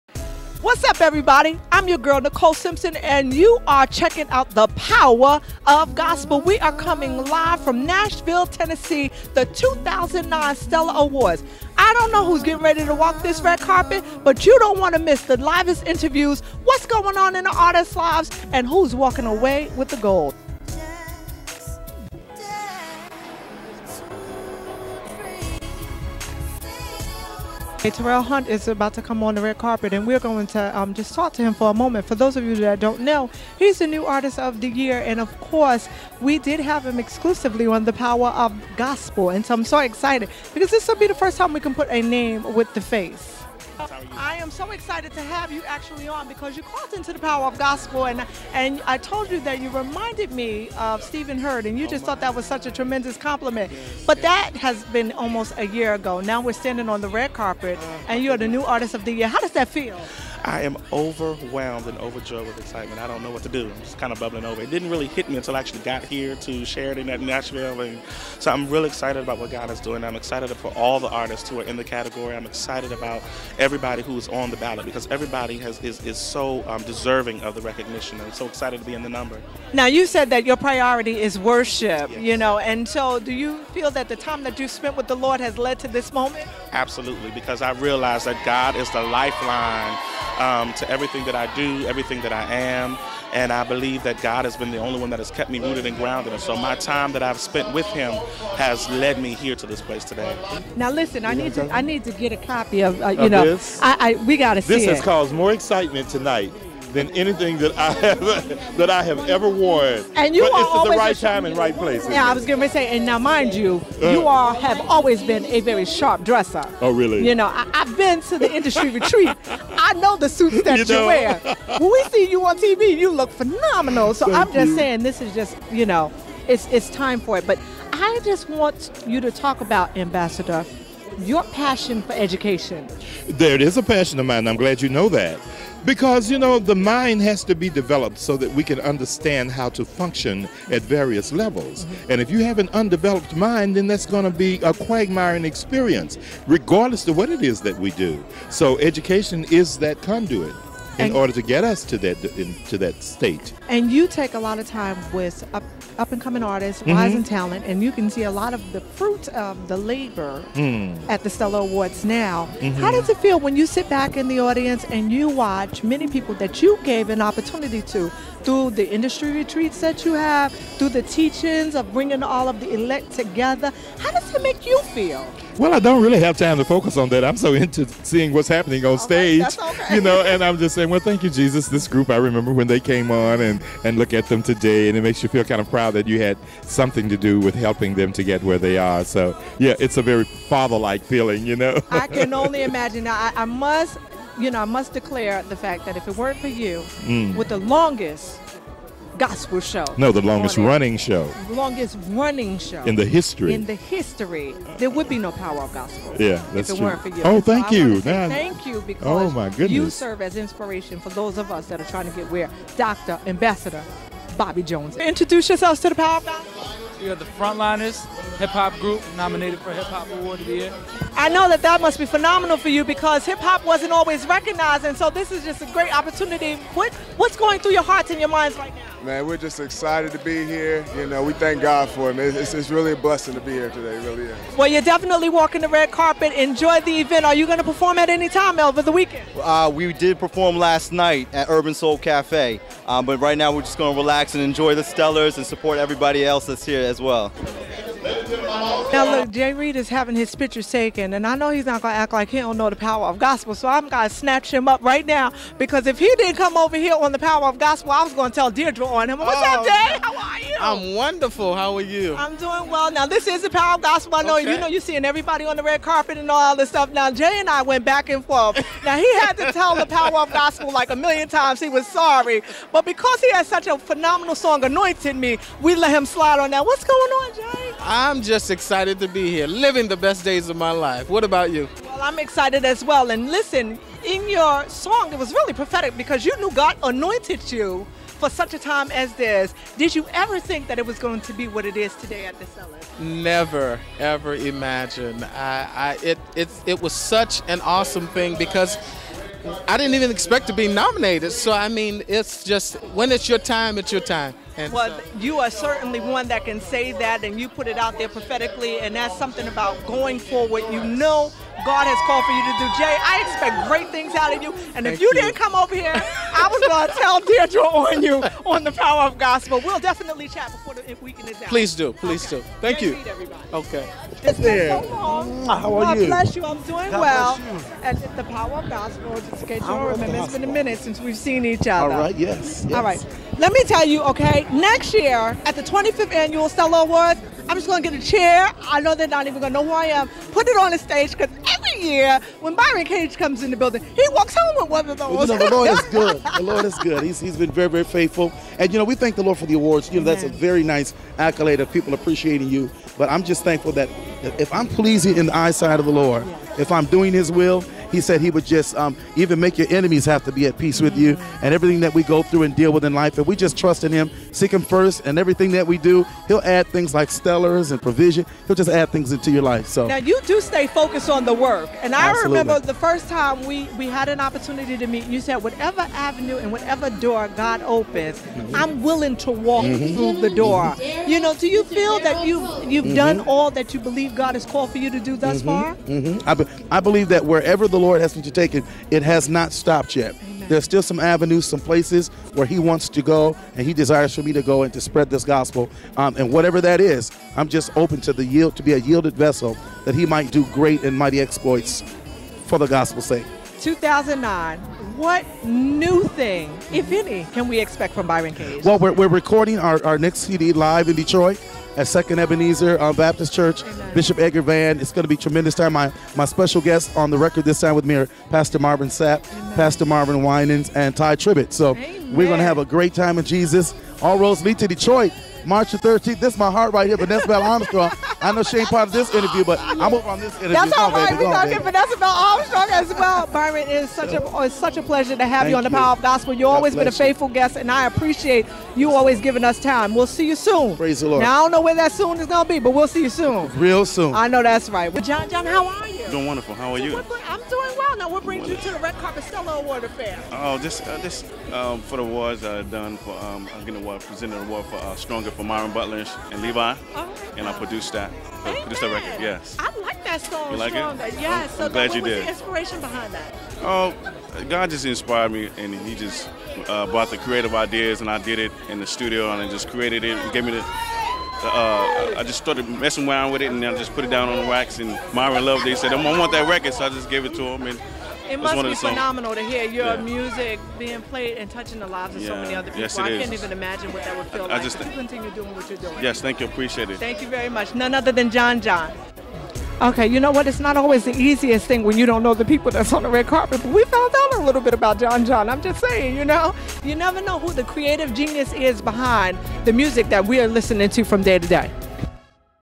Nicole B. Simpson / 2009 Stellar Awards Pt 1
Nicole B. Simpson takes us on the red carpet of the 2009 Stellar Awards.